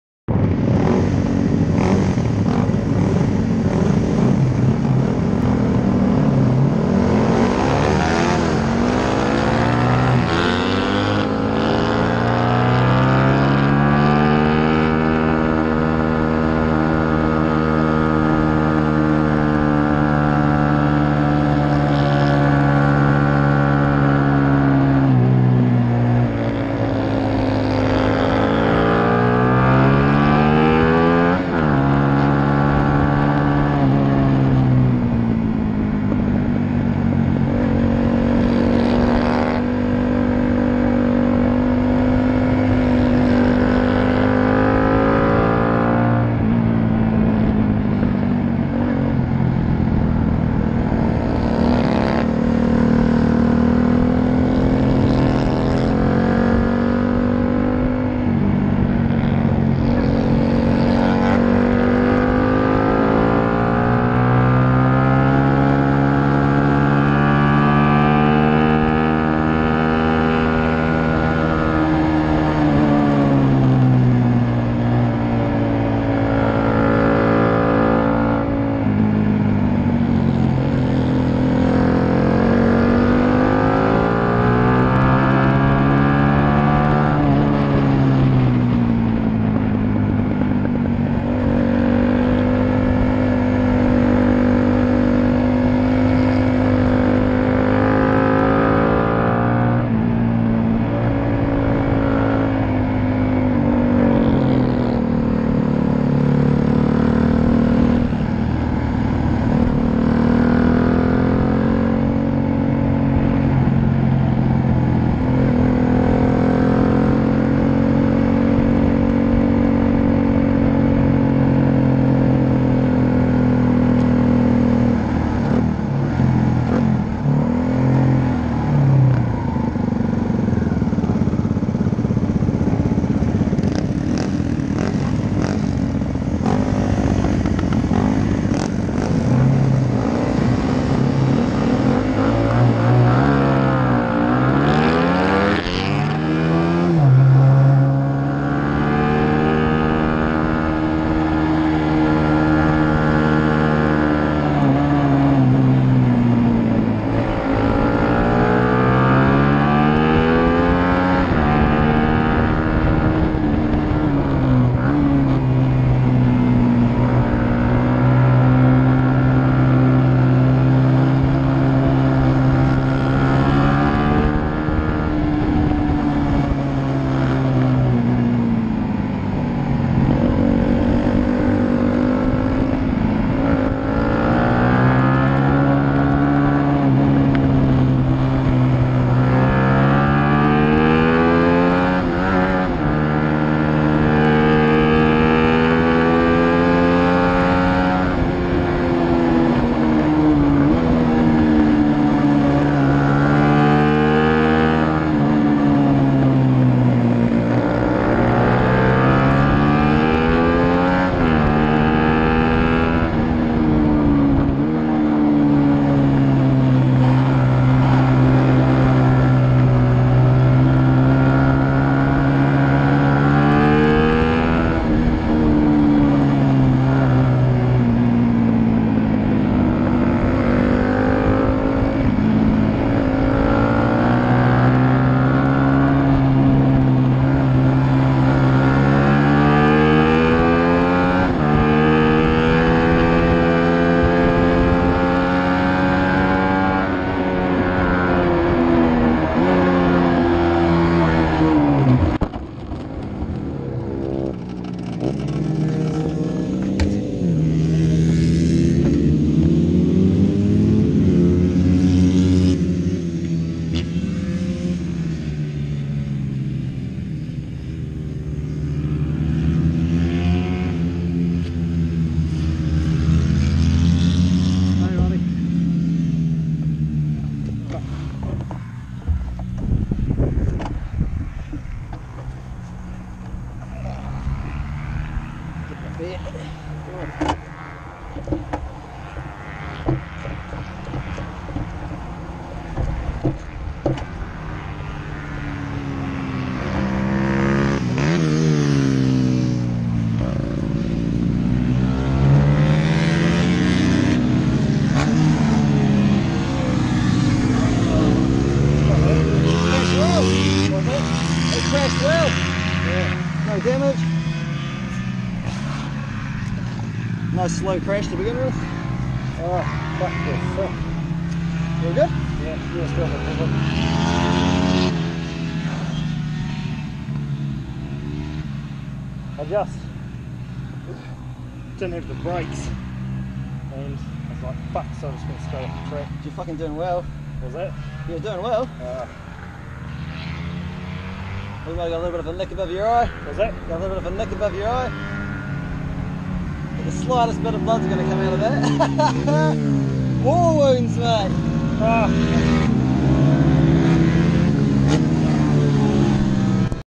kaitoke B grade buckets May club round
losing front end on turn 2,bike came out good and only got a concussion.
looking forward to next meet !